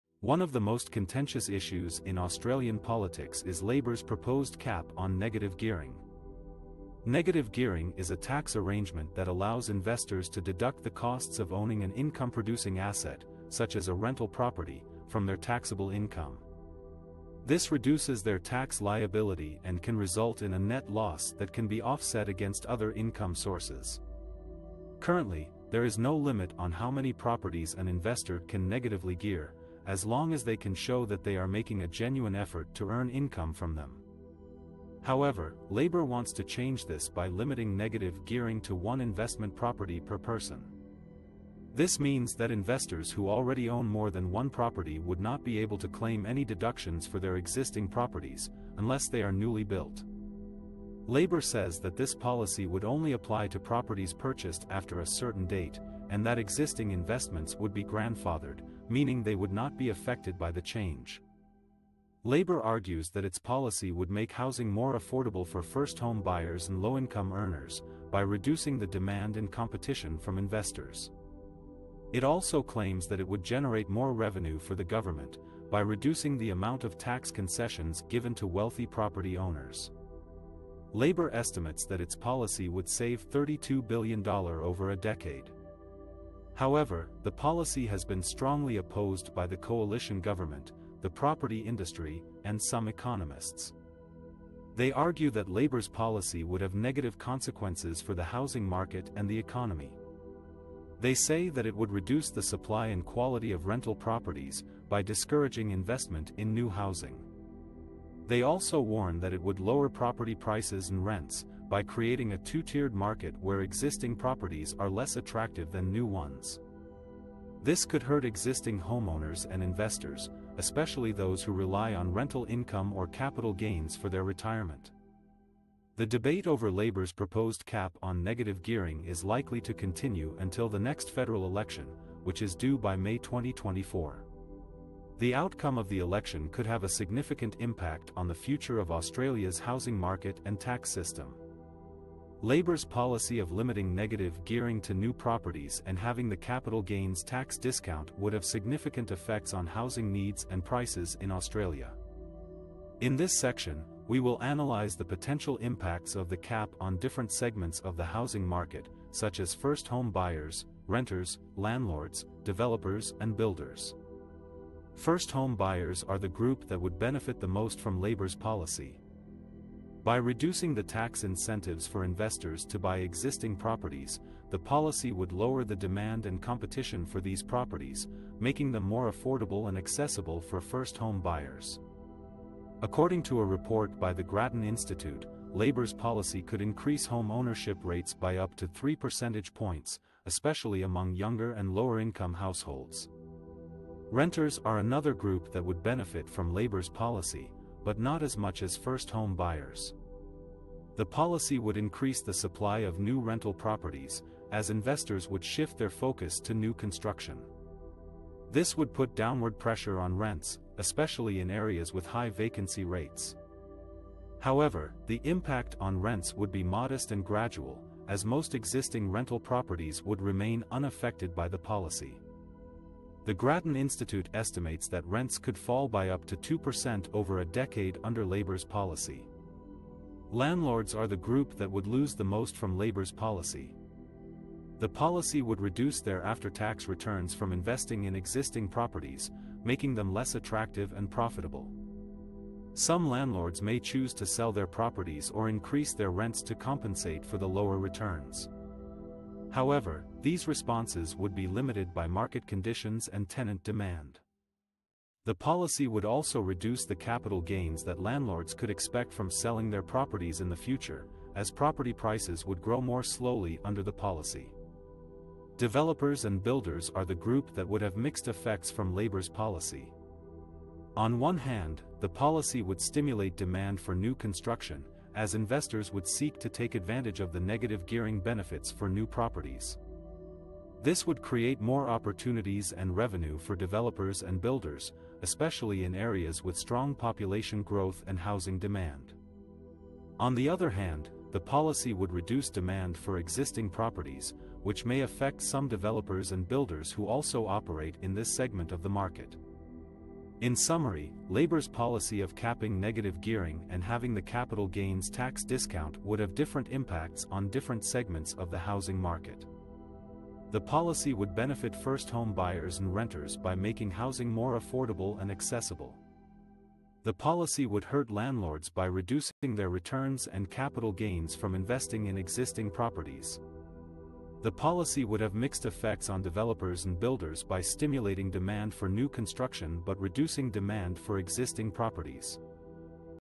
How Labor's Negative Gearing Cap Could Affect Housing Needs and Prices
In this video, we discuss how Labor's negative gearing cap could affect housing needs and prices. Negative gearing is a tax strategy that allows investors to claim losses on their investment properties as a tax deduction. If elected, Labor proposes to limit negative gearing to new housing only. This means that investors can only deduct net rental losses from newly constructed properties from their wage income. We will explore the implications of this policy on the housing market and what it means for renters and homeowners.